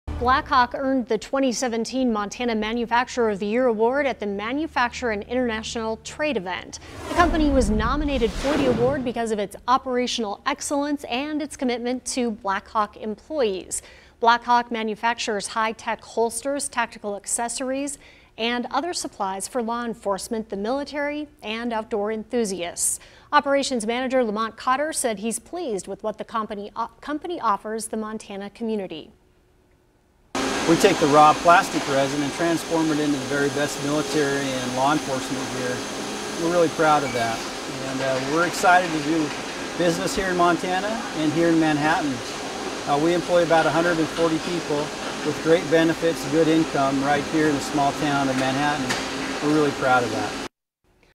Manhattan company tapped as state’s manufacturer of year
BLACKHAWK! in Manhattan earned the 2017 Montana Manufacturer of The Year Award at the recent Manufacture and International Trade event.